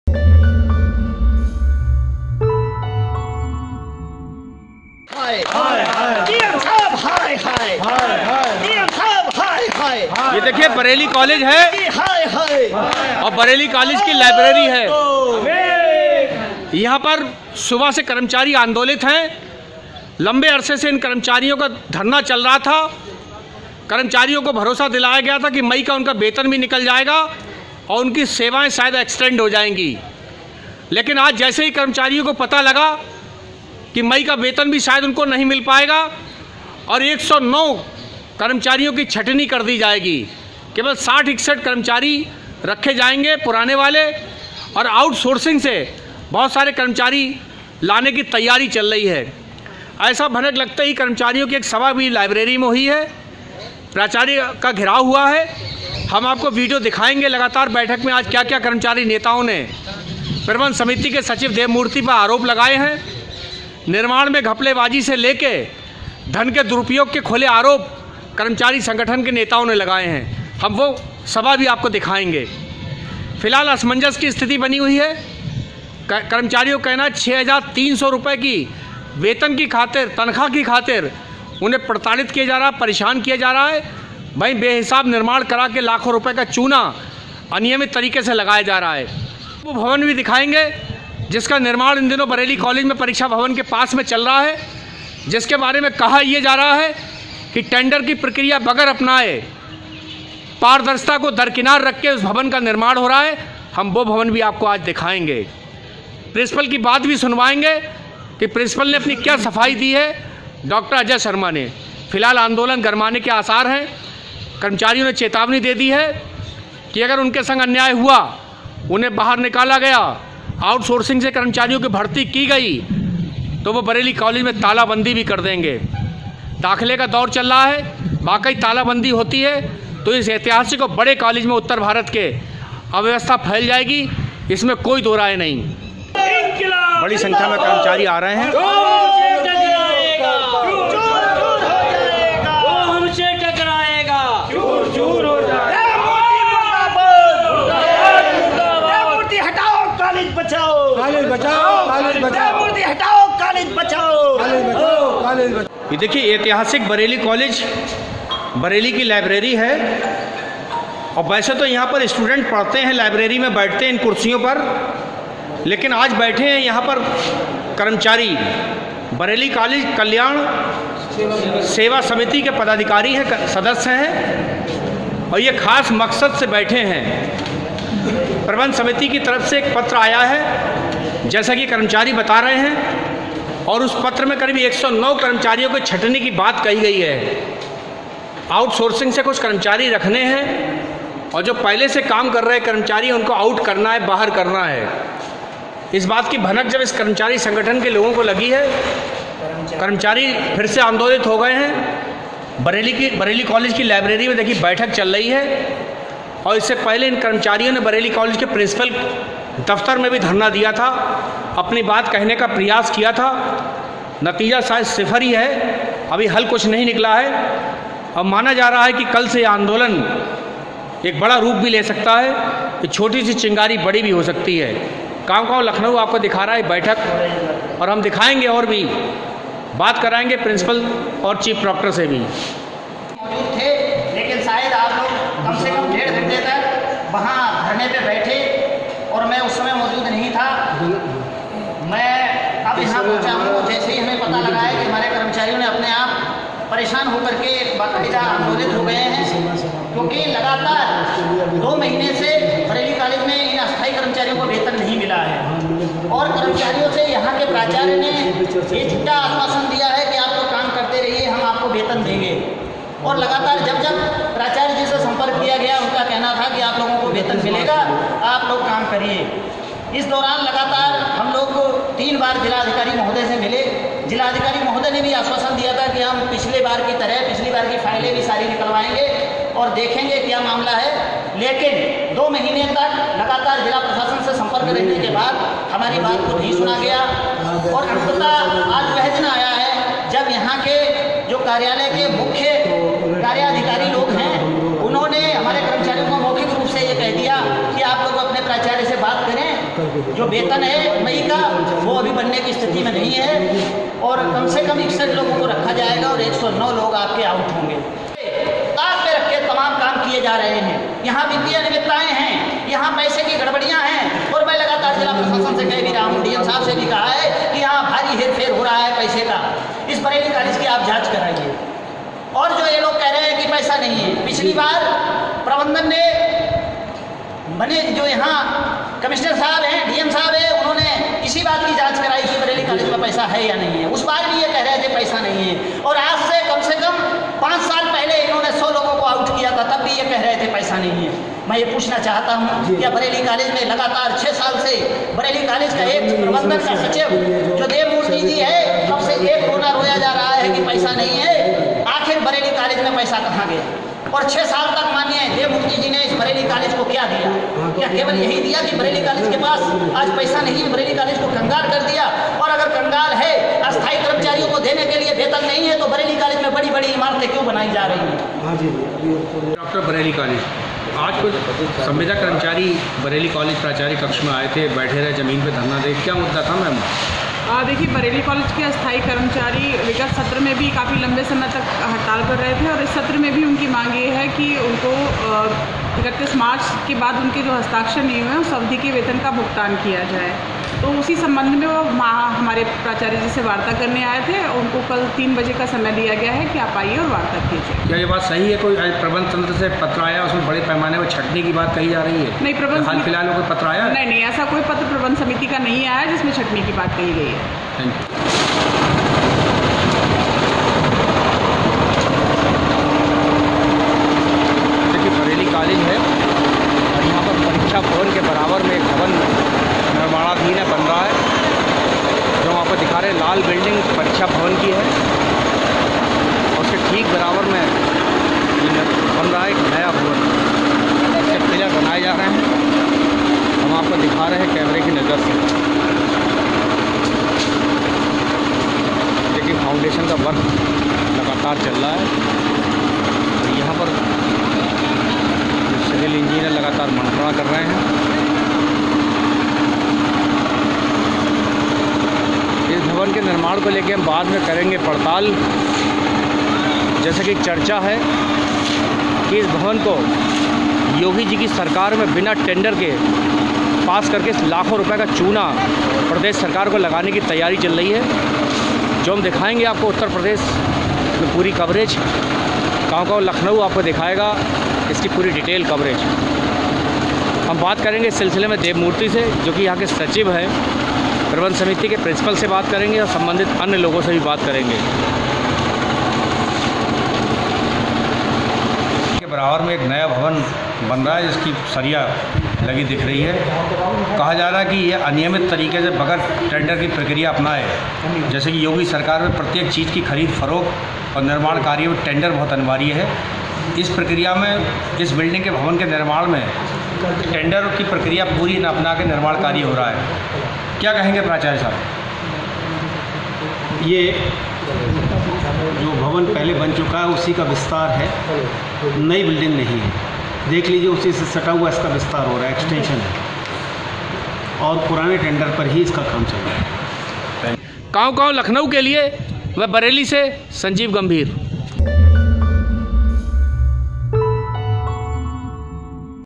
बरेली कॉलेज में फिर भड़का संविदा कर्मचारियों का आंदोलन...
109 कर्मचारियों की छटनी और आउटसोर्सिंग से नये कर्मचारियों की भर्ती की भनक लगते ही भड़के कर्मचारी। प्राचार्य दफ्तर के घेराव के बाद लाइब्रेरी में बैठक की। प्रबंध सचिव देवमूर्ति के खिलाफ नारेबाजी और लगे तमाम आरोप।